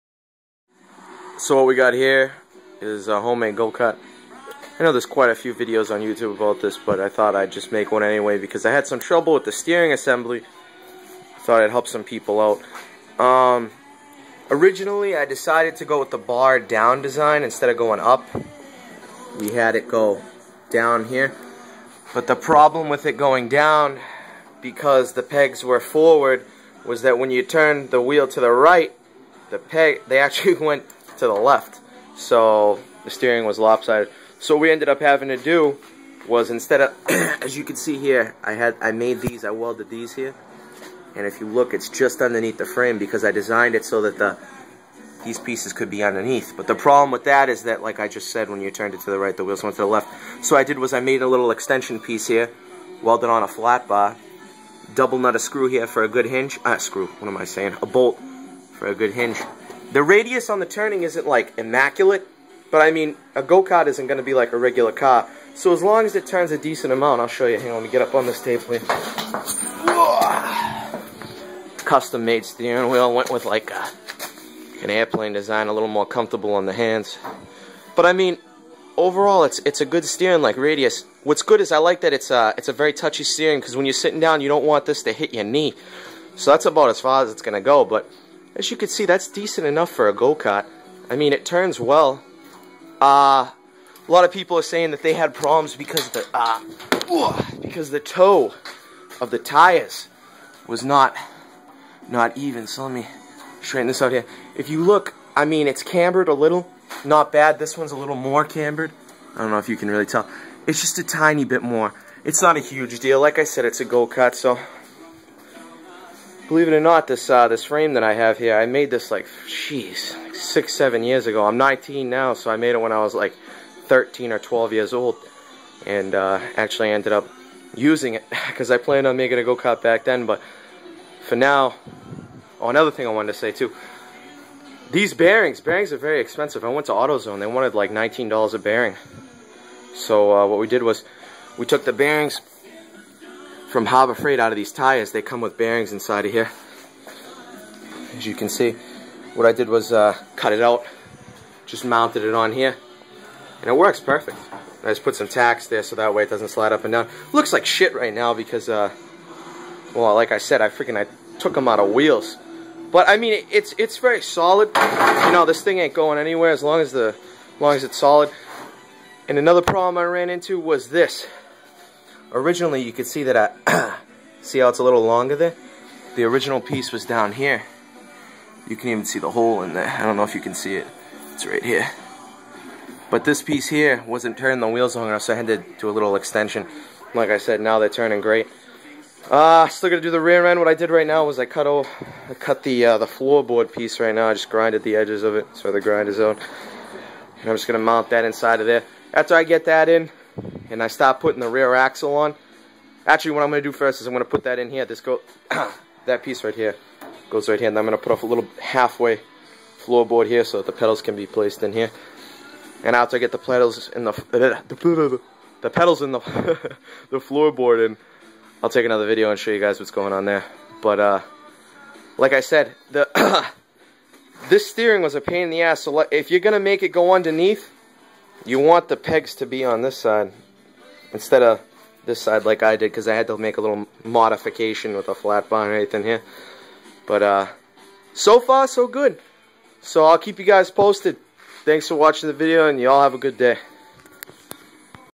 Custom Go-Kart Project "Steering"
This is the first stage of my home made go kart. Using a 6 HP Predator engine from Harbor Freight, 212 cc generating 8 ft/lbs of torque.